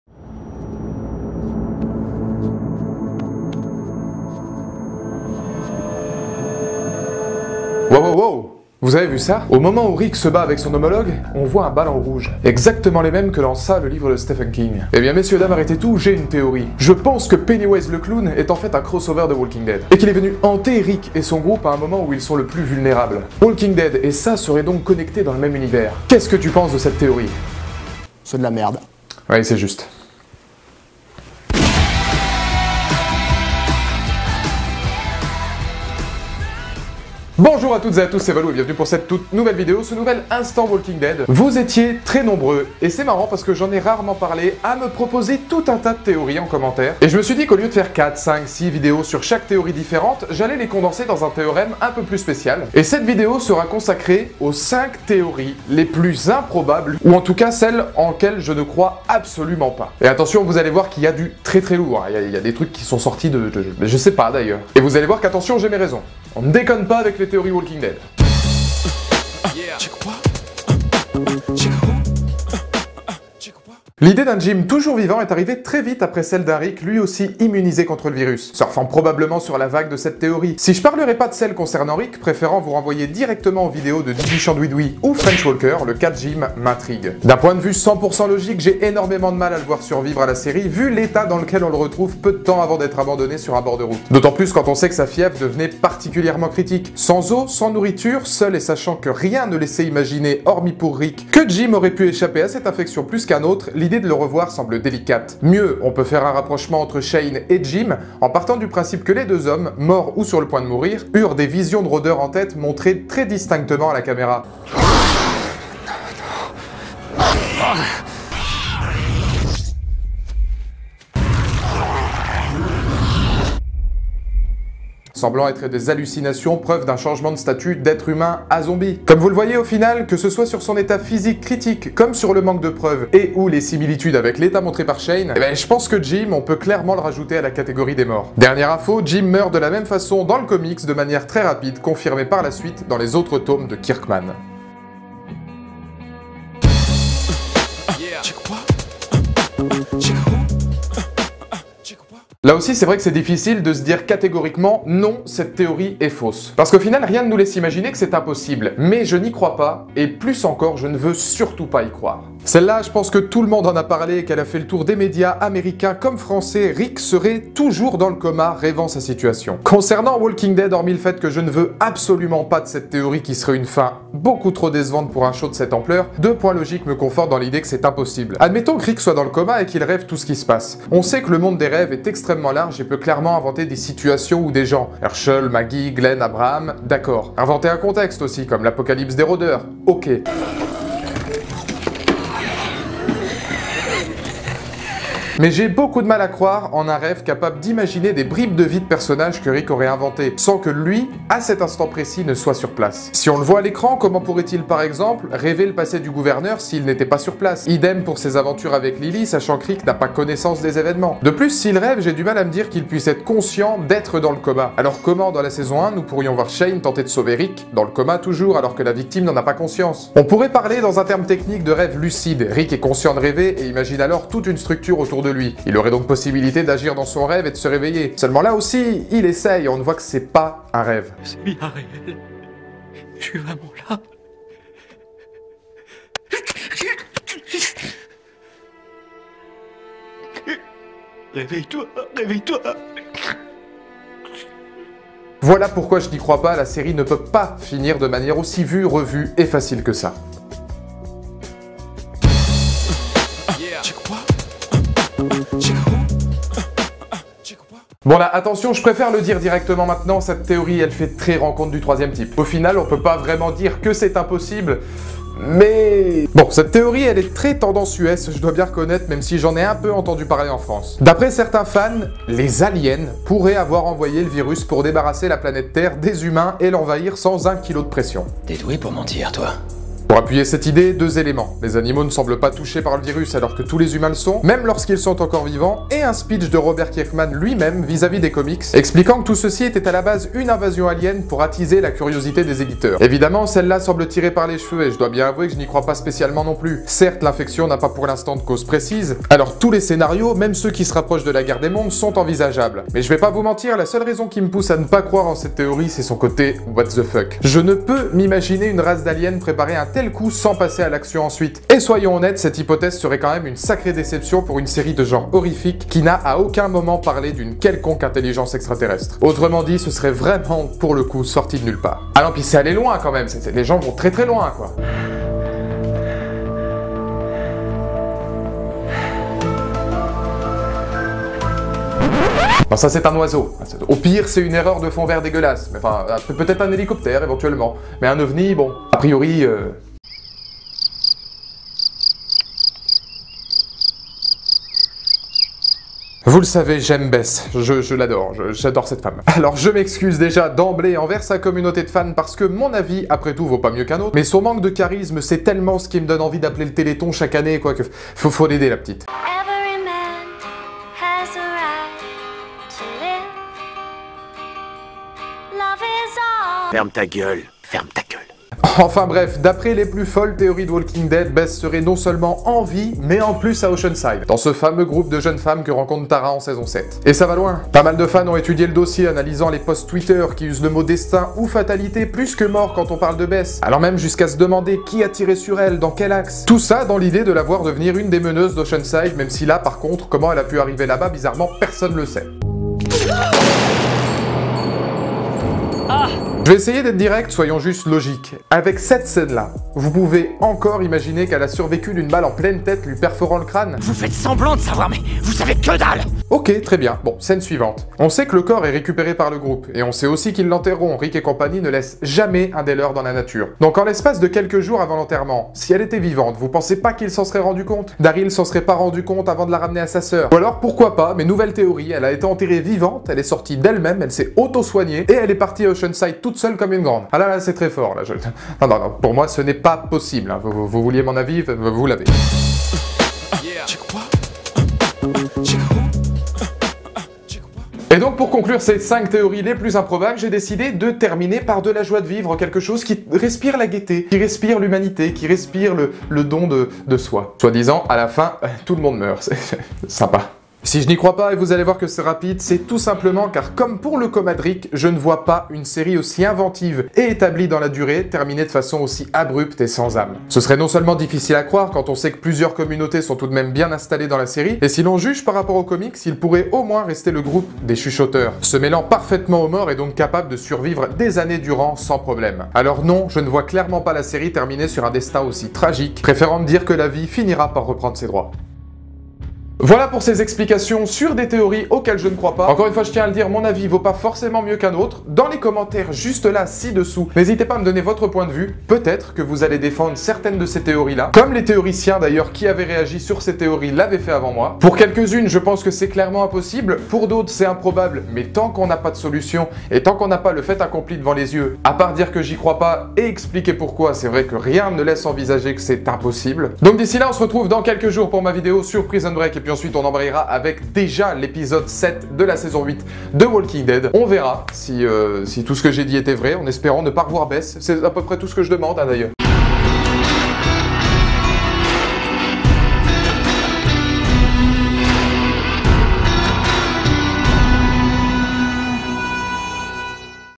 L'Instant "Walking Dead" : 5 Théories Impossibles (Spoils Comics) ?
Avant de se retrouver avec Prison Break dans deux jours, l'heure est aux théories les plus folles de Walking Dead ! Beaucoup me le demandait alors votre hôte s’exécute : après un gros taff de recherche de preuves, voici 5 théories auxquelles je ne crois pas... ou ne veux pas croire !